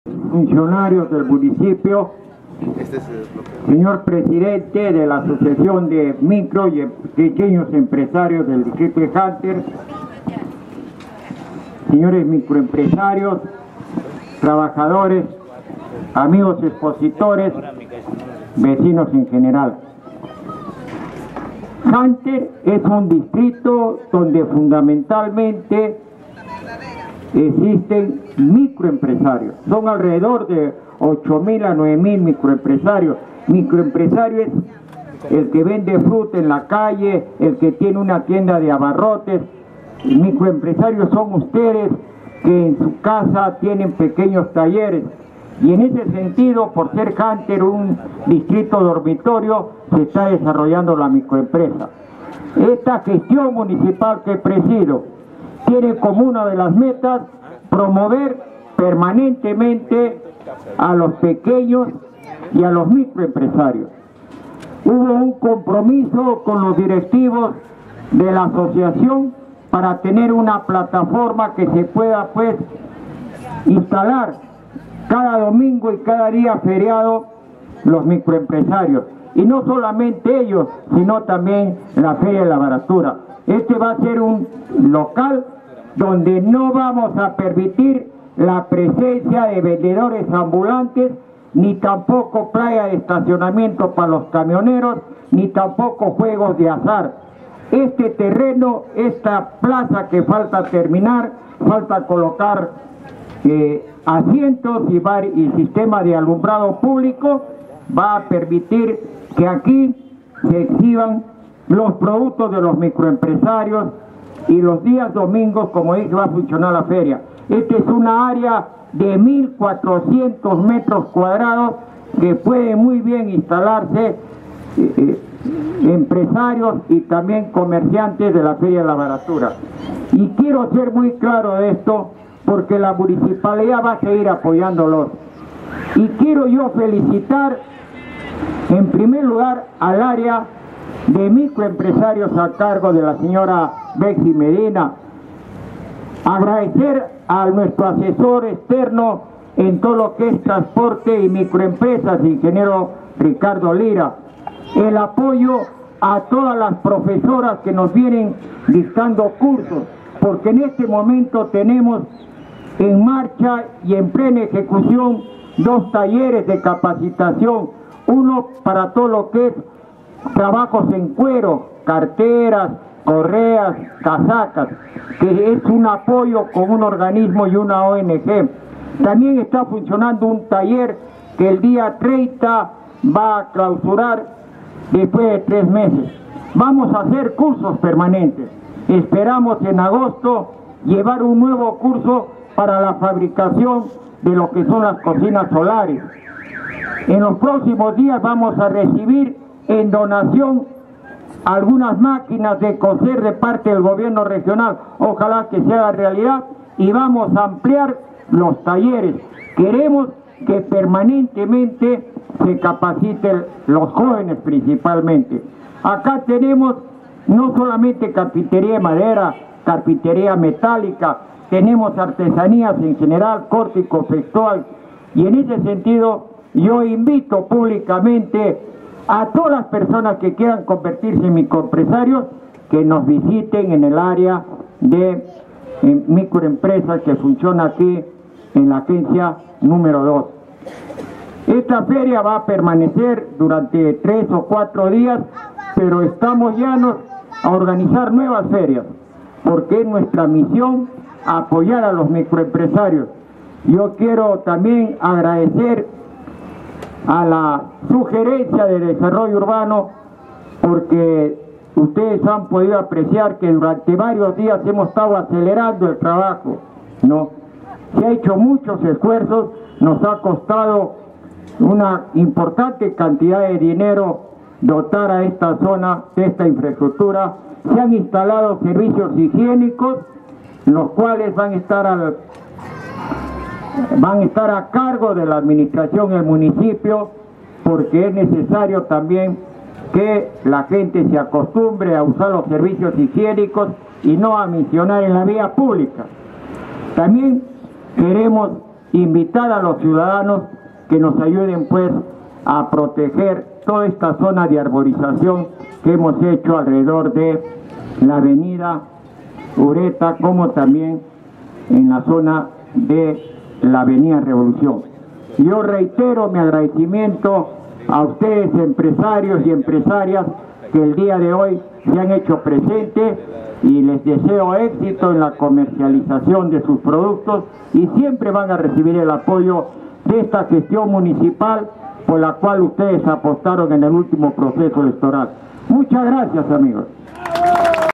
Inaguración de la Feria Empresarial "Bodas de Plata" 2015
Palabras del Alcalde de Hunter don Simón Balbuena Marroquín durante la Inaguración de la Feria Empresarial "Bodas de Plata" 2015, organizado por la Municipalidad Distrital Jacobo Hunter y la Asociación de Micro y Pequeños Empresarios de Hunter (AMPEH),
del 27 al 29 de Junio en el cruce de las Av. Mcal. Ureta y Revolución